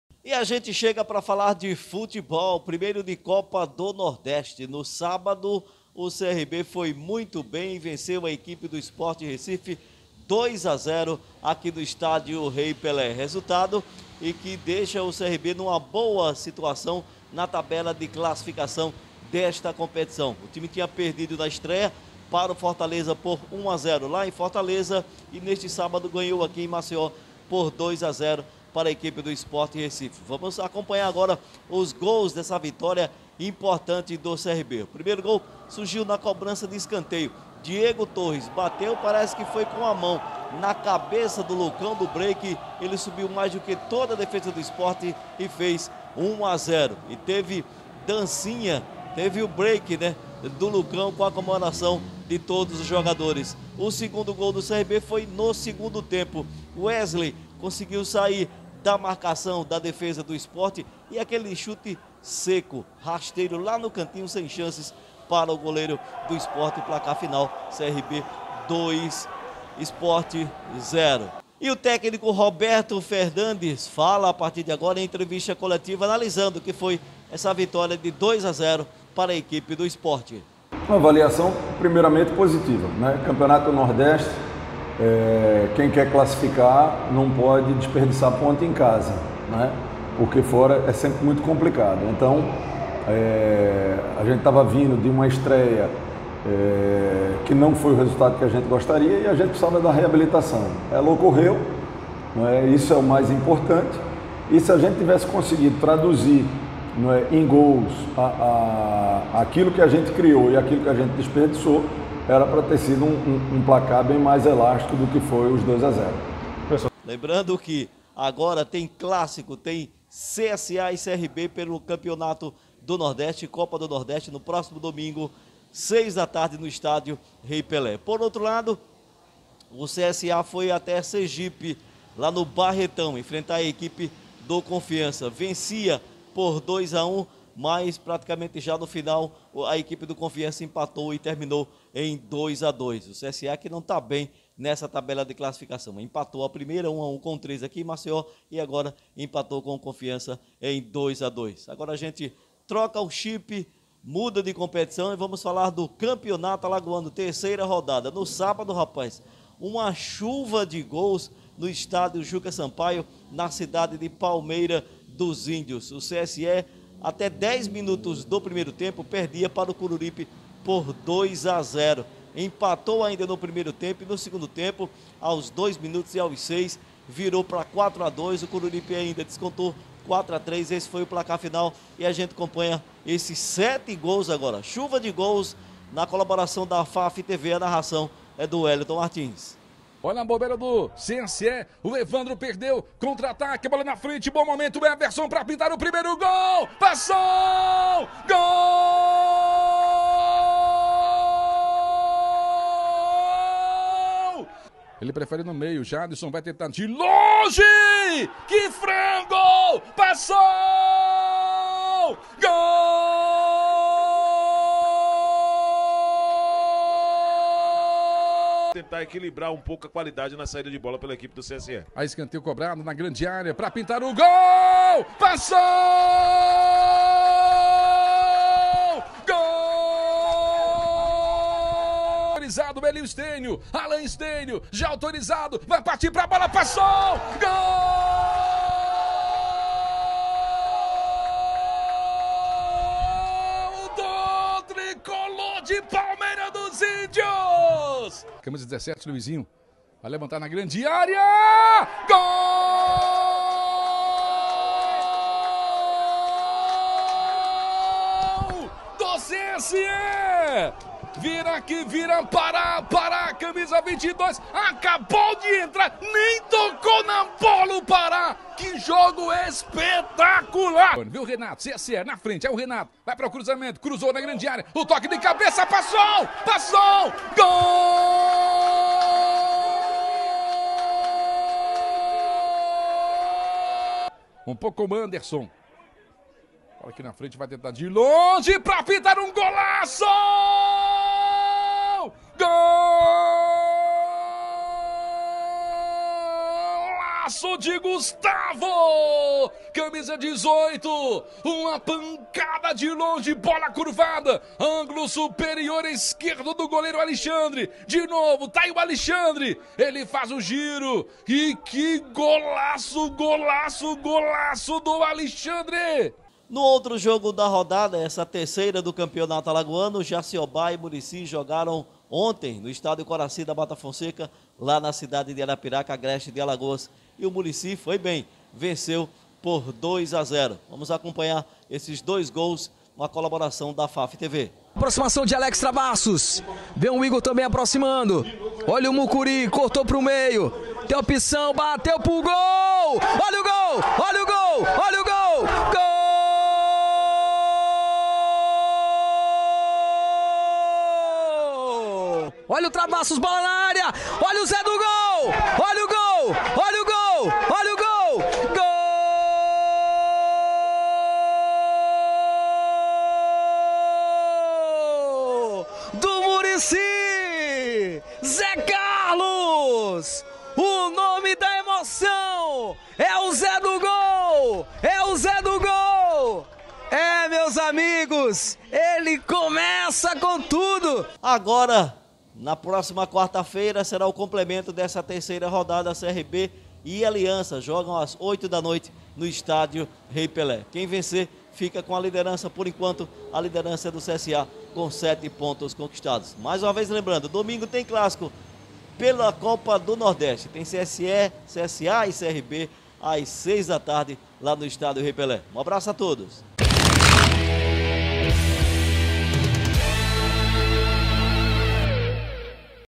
CAMPEONATO ALAGOANO: CHUVA DE GOLS NO ÚLTIMO FINAL DE SEMANA
Alberto Lima traz os gols do Campeonato Alagoano e da Copa do Nordeste do último final de semana. CRB vence por 2 x 0 contra o Sport pela Copa do Nordeste. O CSA empatou com o Confiança em 2 x 2. E no Campeonato Alagoano, Cse venceu o Coruripe por 4x3, e o Murici venceu o Jaciobá por 2x0.